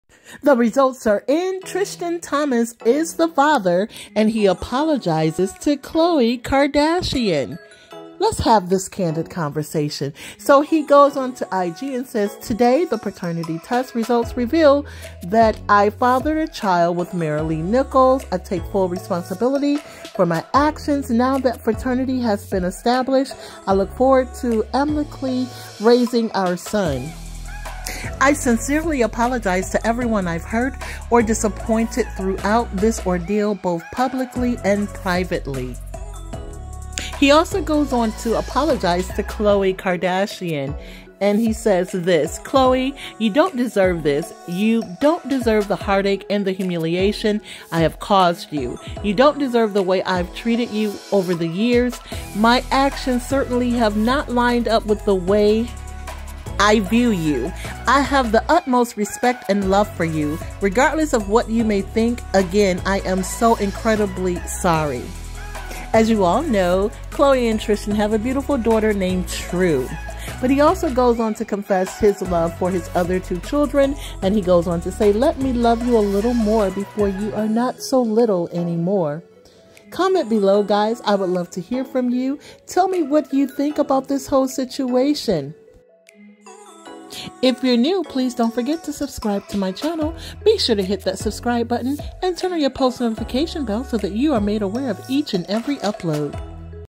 TRISTIAN THOMPSON IS THE FATHER & HE APOLOGIZES TO KHLOE KARDASHIAN
Hidn Treasure Designs Jewelry